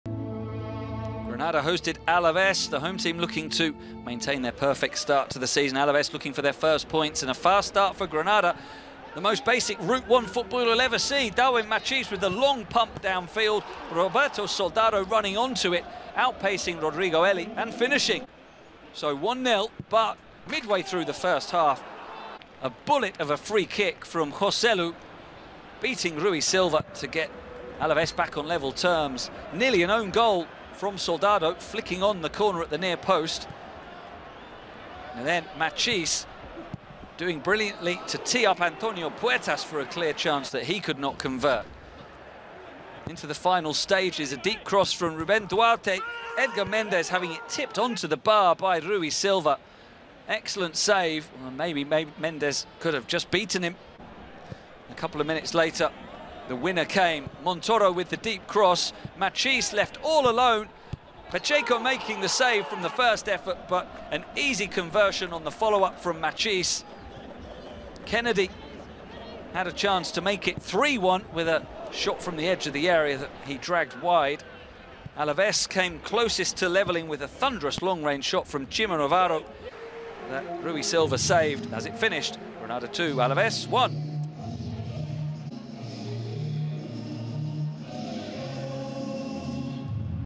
Highlights Granada CF vs Deportivo Alavés (2-1)
Granada CF defeat Deportivo Alavés withe the goals of Soldado and Machís and mantain a perfect start to the season GranadaAlavés. Matchday 02 LaLiga Santander 2020/2021